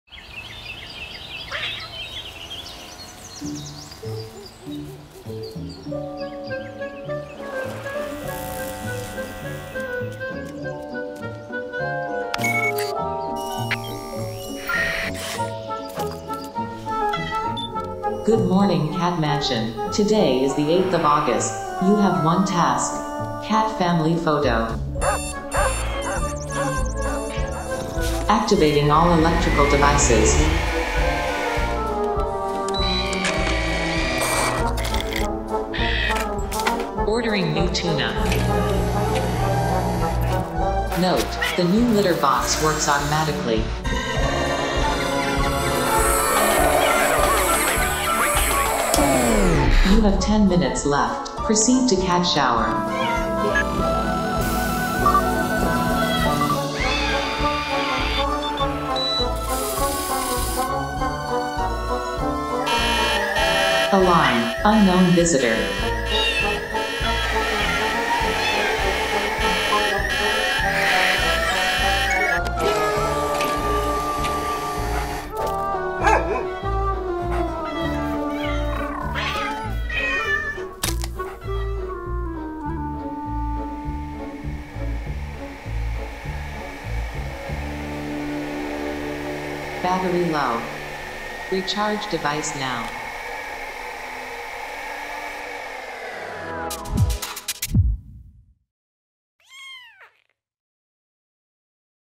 Cat Mansion
What happens when we provide cats with smart home energy products? We tested this out and gave the cats something to work with on International Cat Day. Even the kittens are taken care of and everyone has a full belly before the big family photo is taken.

A special thanks to the team at videoboost.
Our director & story creator: Nils Keller
Director of Photography: Georg Nikolaus